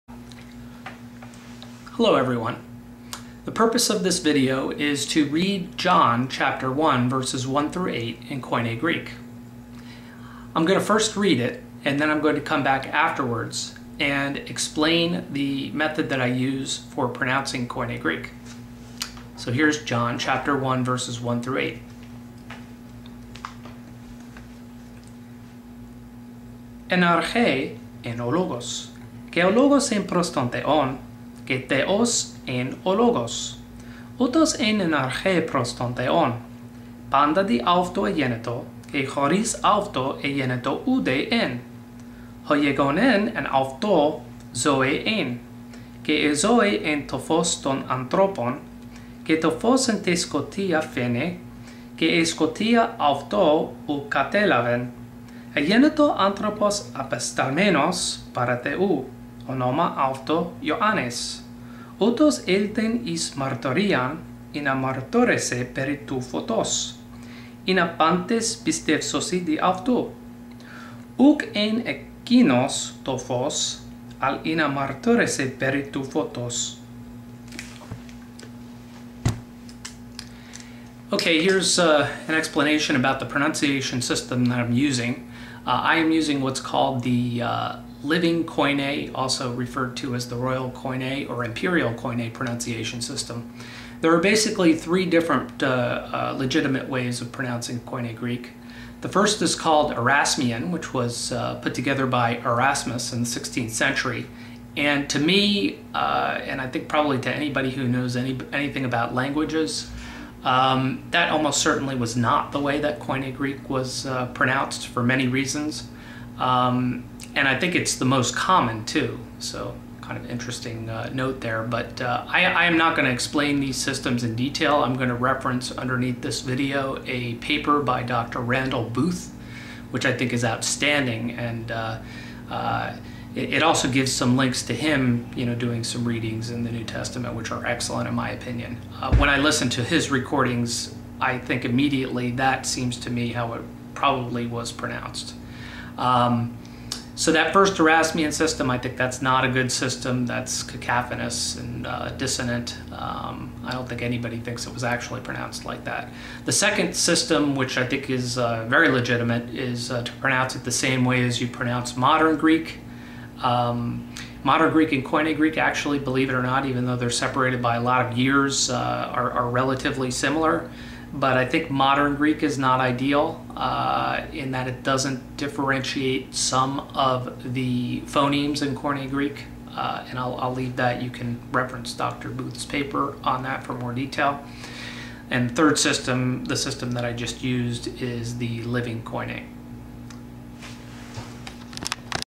John 1:1-8 in Living Koine Greek
A reading of John 1:1-8 in Koine Greek using the Living Koine Pronunciation System. An excellent paper about pronouncing Koine Greek, as well as some readings by the author of the paper, Dr. Randall Buth, is available here:

A transcript of the text read follows:

 1εν αρχη ην ο λογος και ο λογος ην προς τον θεον και θεος ην ο λογος
 2ουτος ην εν αρχη προς τον θεον
 3παντα δι αυτου εγενετο και χωρις αυτου εγενετο ουδε εν ο γεγονεν
 4εν αυτω ζωη ην και η ζωη ην το φως των ανθρωπων
 5και το φως εν τη σκοτια φαινει και η σκοτια αυτο ου κατελαβεν
 6εγενετο ανθρωπος απεσταλμενος παρα θεου ονομα αυτω ιωαννης
 7ουτος ηλθεν εις μαρτυριαν ινα μαρτυρηση περι του φωτος ινα παντες    πιστευσωσιν δι αυτου

    8ουκ ην εκεινος το φως αλλ ινα μαρτυρηση περι του φωτος